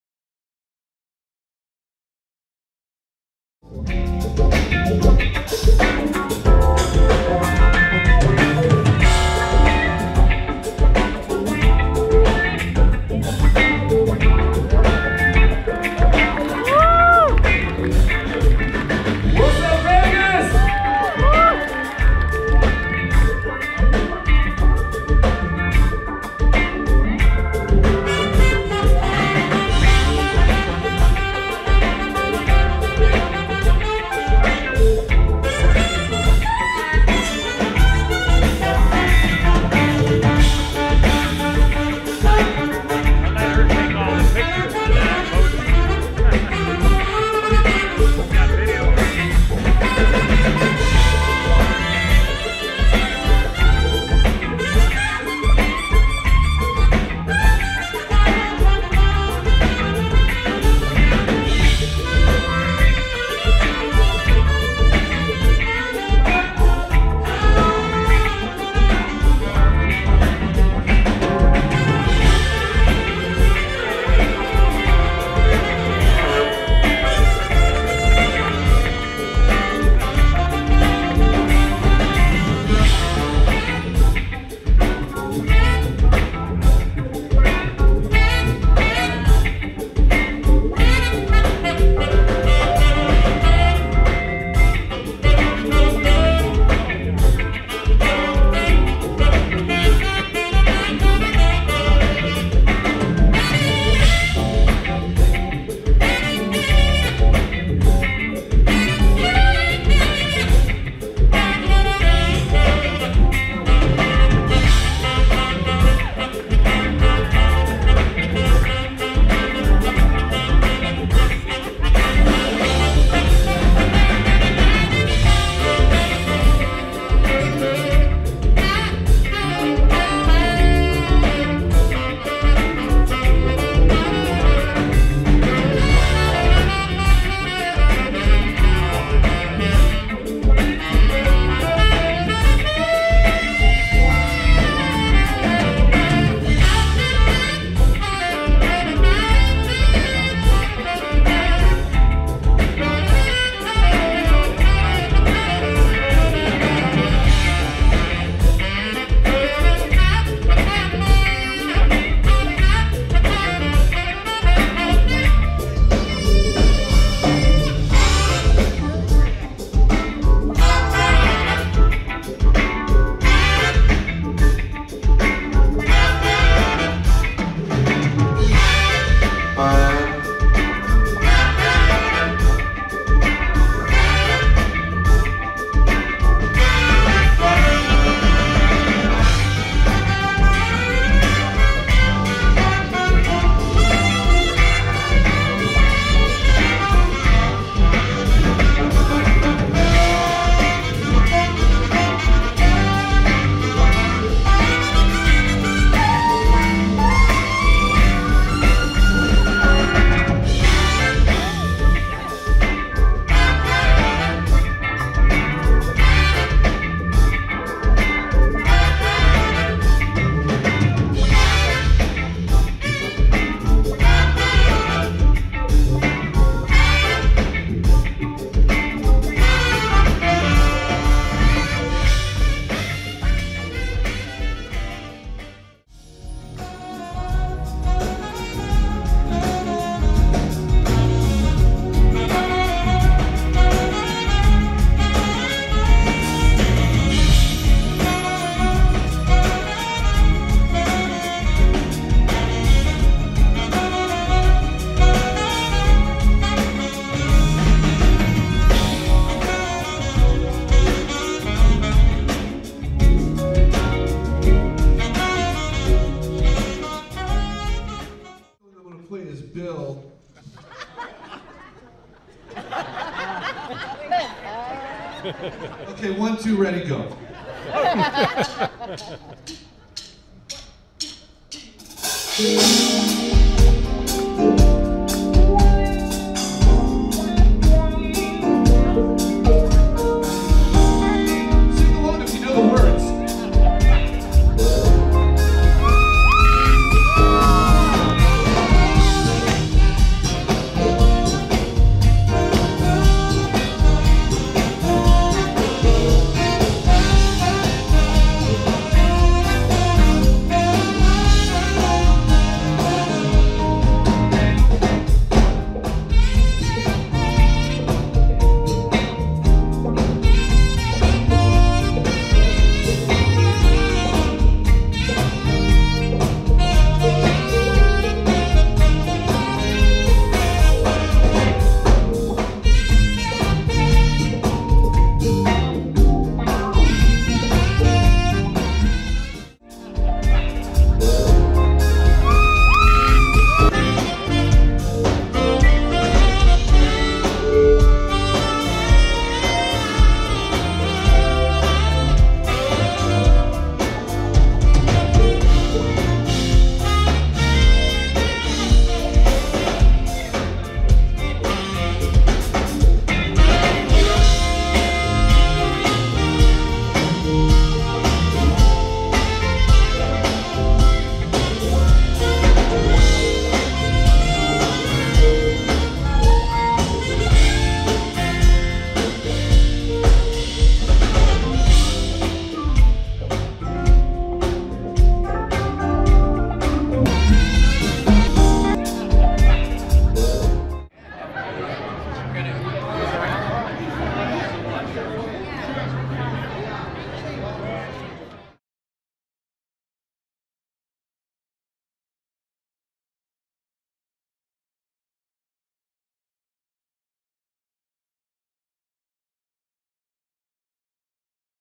Sax Pack Live at Aliante Hotel & Casino Las Vegas, NV 2016
A Wonderful concert featuring the great  music of one the the legends of Jazz Music - The Sax Pack - featuring  Jeff Kashiwa, Steve Cole,  Marcus Anderson. A must see concert in a small 300 seat venue at the Aliante Hotel & Casino